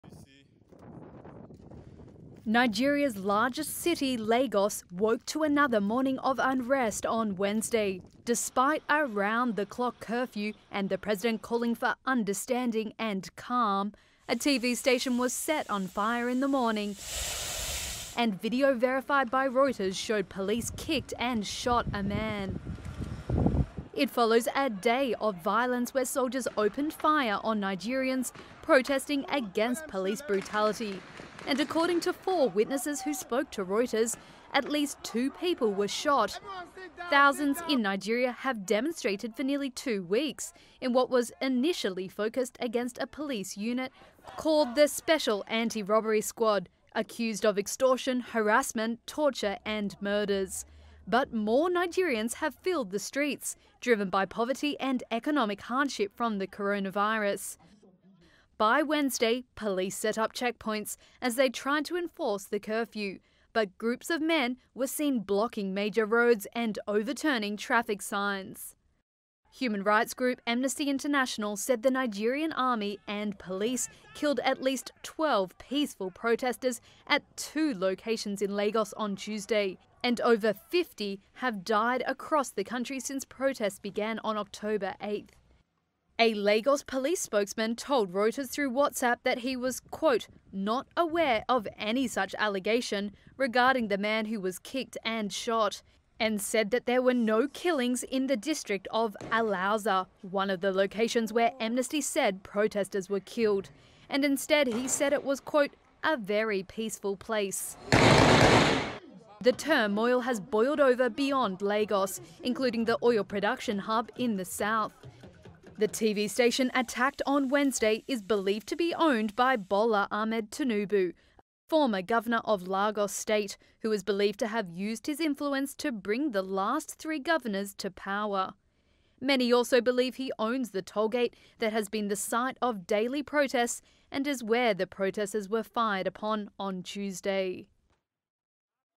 Fires in Nigeria after soldiers shoot protesters
Nigeria's biggest city Lagos and several states were under curfews as unrest rooted in anti-police protests broke out again following a day of violence, including the shooting of civilians by security forces.